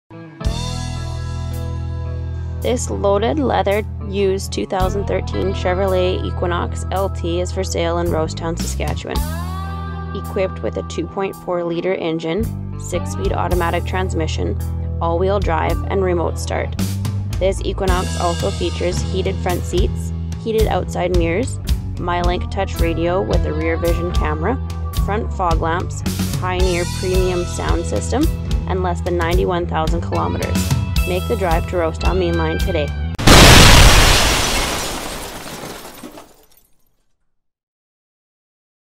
60093 2013 CHEVY EQUINOX 2LT All-Wheel Drive for sale in Saskatchewan
This loaded, leather, used 2013 Chevy Equinox LT for sale in Saskatchewan on our Mainline Motors GMC, Buick, and Chevrolet lot, is ready to go! Equipped with a 2.4L engine, 6-speed automatic transmission, all-wheel drive and remote start! This Equinox also features heated front seats, heated outside mirrors, MyLink touch radio with rear vision camera, front fog lamps, Pioneer premium sound system, and less than 91,000 km! Make the drive to Rosetown Mainline Motors, the ORIGINAL King Of Trucks, today!